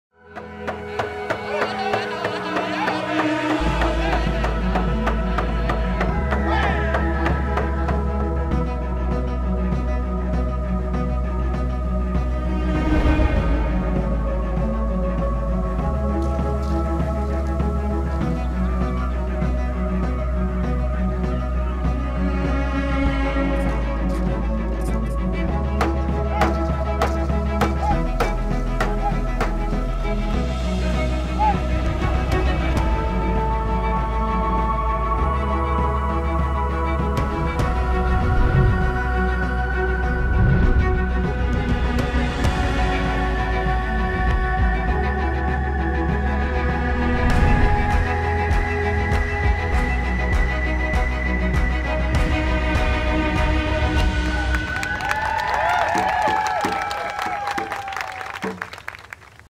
Native American Festival Hoop Dancing
Created a small highlights video for a the annual Native American Festival happening downtown at Shoreline Village.